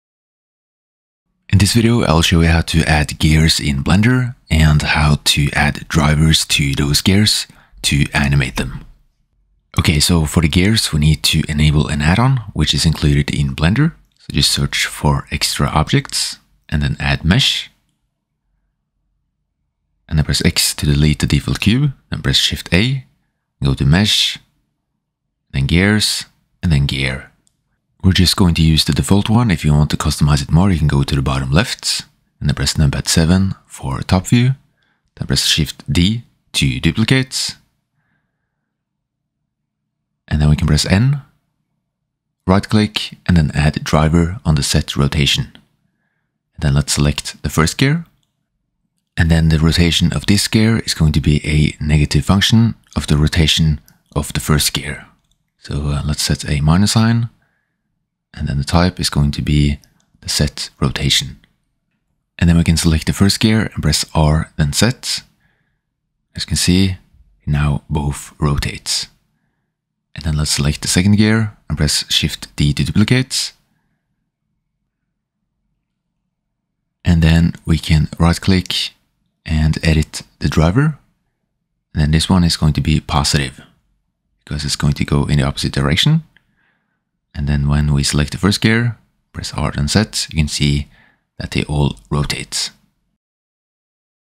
[4.0] Blender Tutorial: Quick Gears And Drivers, Beginner Tutorial
Learn how to use drivers in Blender using gears.